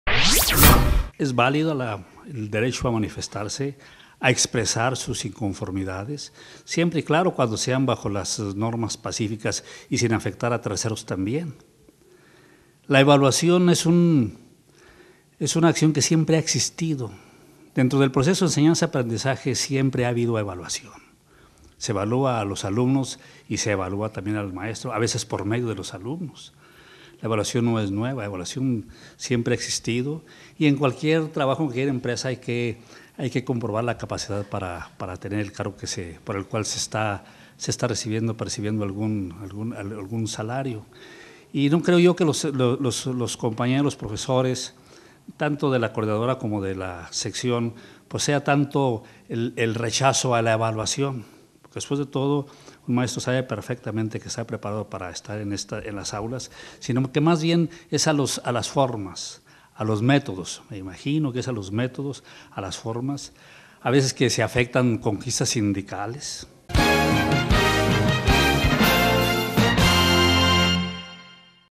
Es válido el derecho de los maestros a manifestarse.
Los maestros tienen todo el derecho a manifestarse, siempre y cuando lo hagan sin afectar a terceros, así lo señaló el dirigente del partido Duranguense Raúl Irigoyen Guerra.